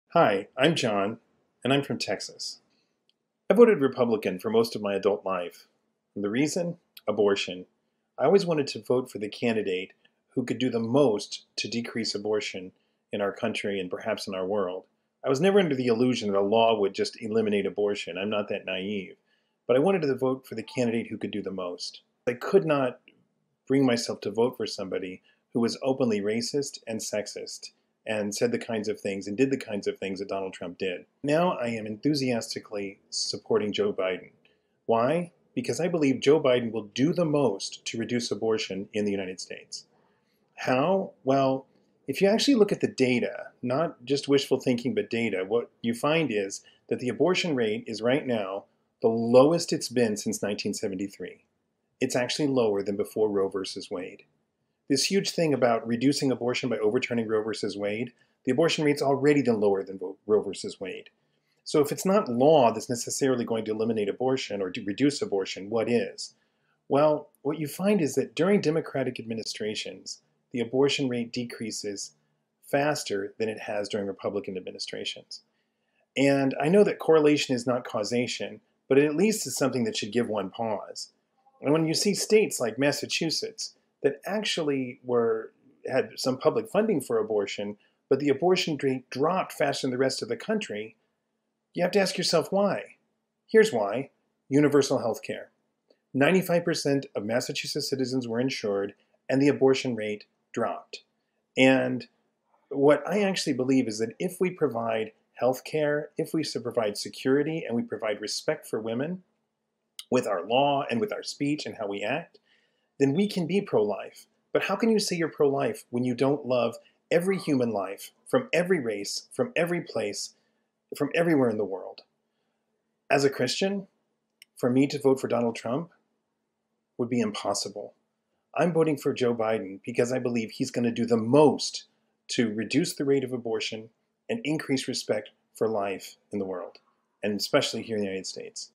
Lifelong Republican: Abortion Rate Decreases Faster During Democratic Administrations
"During Democratic administrations, the abortion rate decreases faster than it has during Republican administrations."